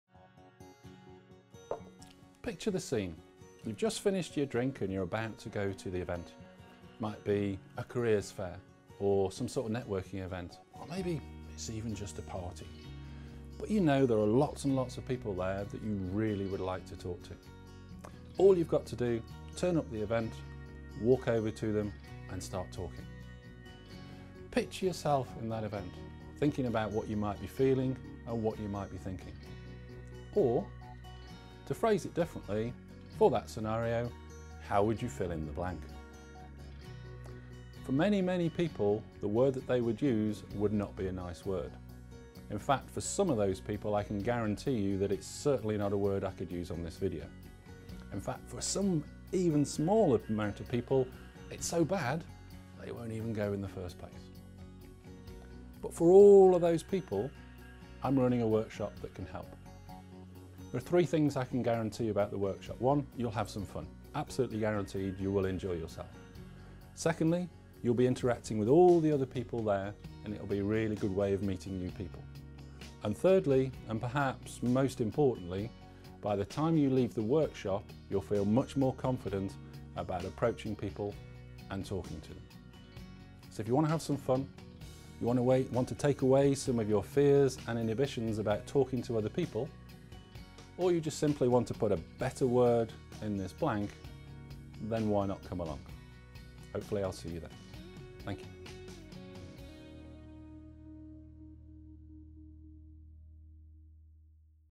CareersFest 2016 Workshop Invitation
Mike Grocott, Director at Intecogm, invites you to attend the networking workshop he is hosting as part of CareersFest 2016